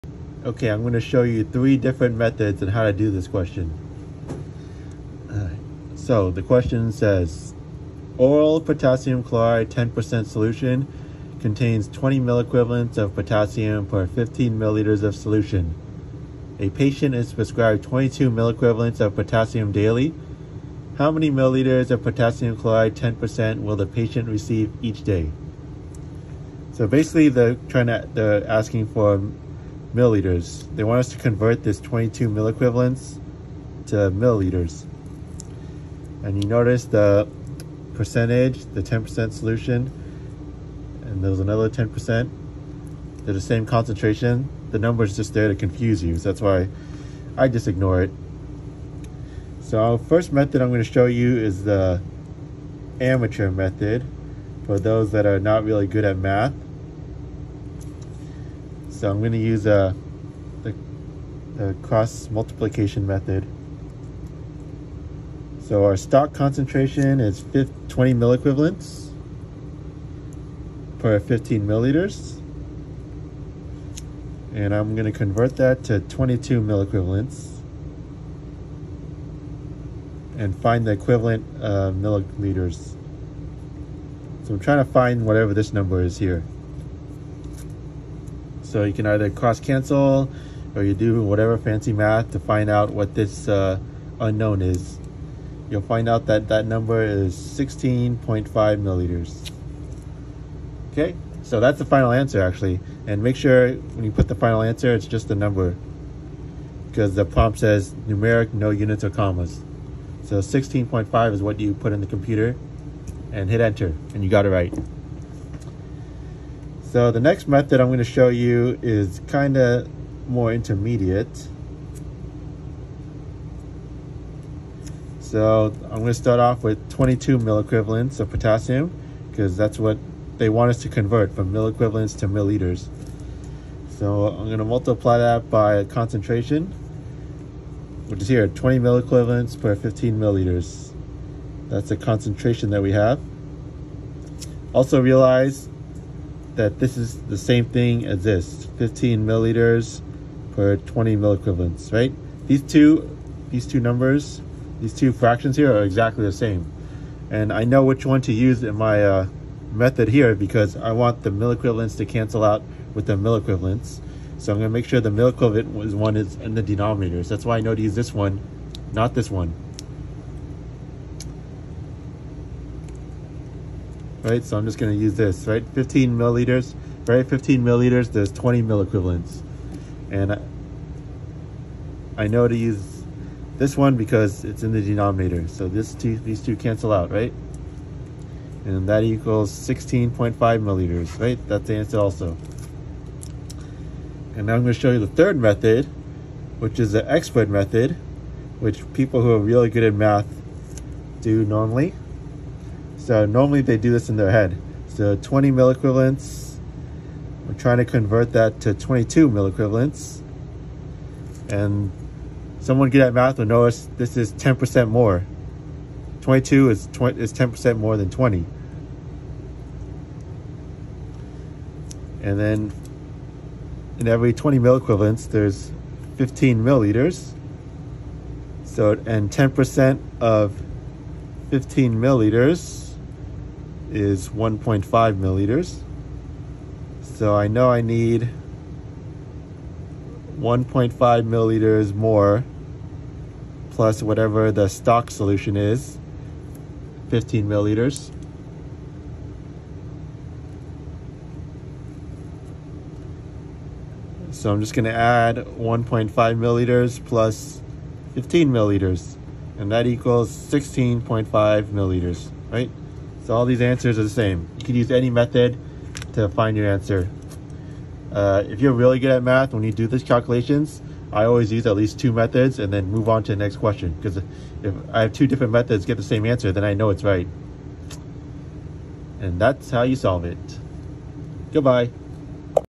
Naplex calculation (level difficulty: easy) - mEq to mL
Oral potassium chloride 10% solution contains 20 mEq of potassium per 15 mL of solution. A patient is prescribed 22 mEq of potassium daily. How many milliliters of potassium chloride 10% will the patient receive each day? (Answer must be numeric; no units or commas.)